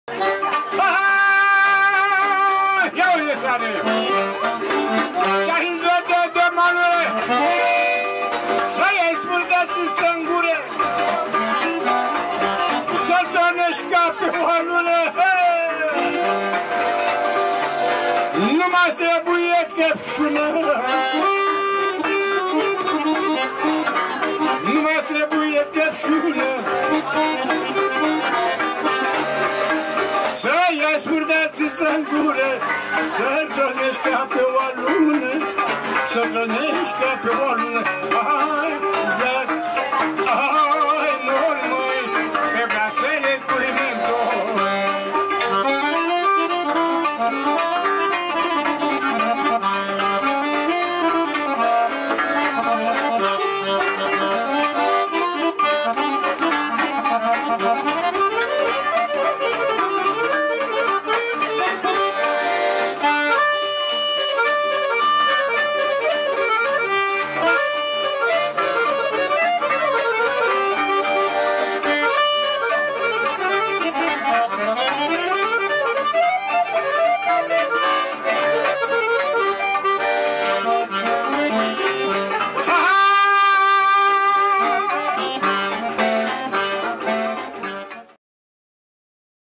Țagoi din Clejani și Shaun Williams B // 14.05.2017
Gurist și acordeonist de excepție, Marin “Țagoi” Sandu provine dintr-o dinastie de lăutari renumiți și apreciați din Clejani. Cântând la vioară și la bas de la vârsta de 14 ani, el și-a descoperit iubirea pentru acordeon mai târziu, abia la 18 ani. Împreună cu tatăl sau, celebrul violonist Nicolae “Culae” Neacșu, Țagoi a facut parte din colectivul inițial ce a format fenomenul Taraf de Haïdouks.

Shaun Williams vine din Statele Unite și se află în România pentru a studia folclorul, muzica lăutărească și manelele.